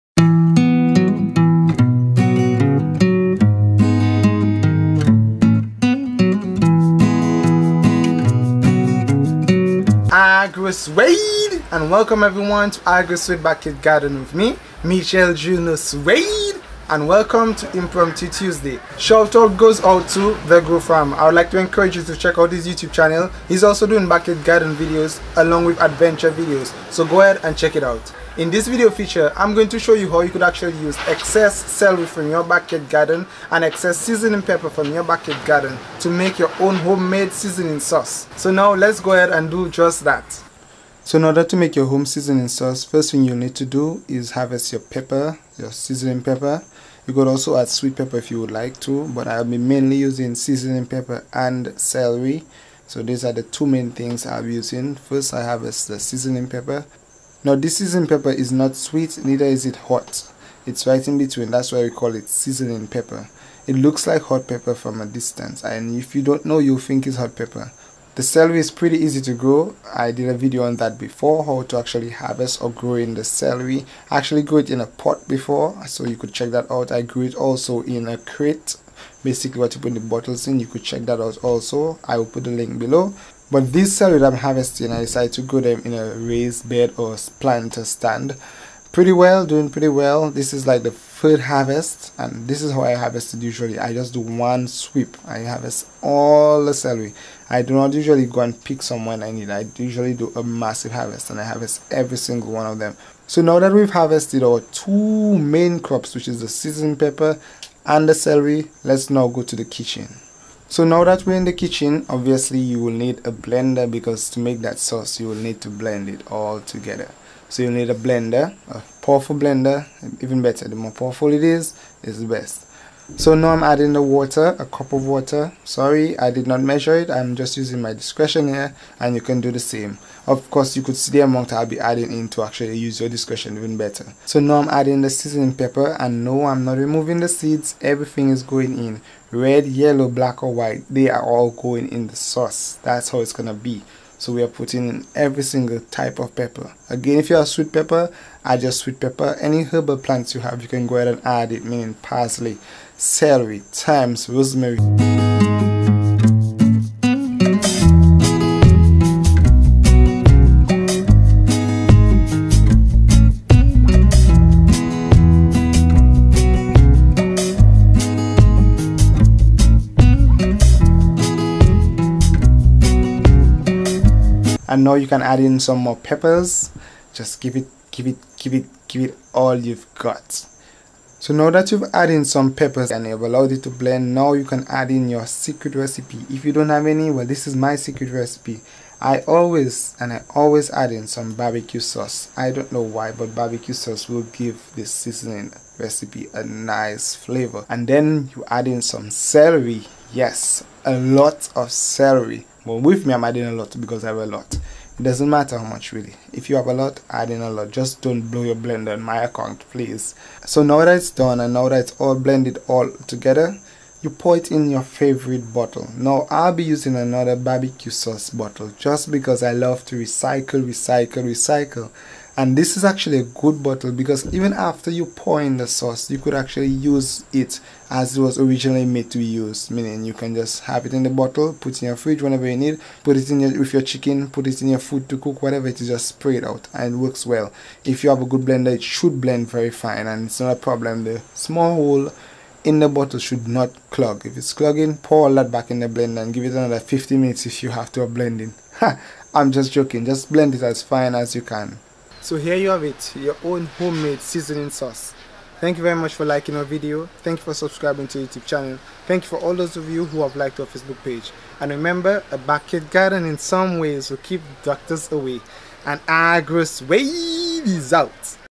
How To Make Green Seasoning | Peppers | Celery
Welcome to Impromptu Tuesday!

How To Make Homemade Green Sauce | Peppers | Celery.

Have you ever had excess pepper, and celery in your backyard garden and you do not know what to do with it?

Well now you can make your own homemade green sauce. Check out this video feature!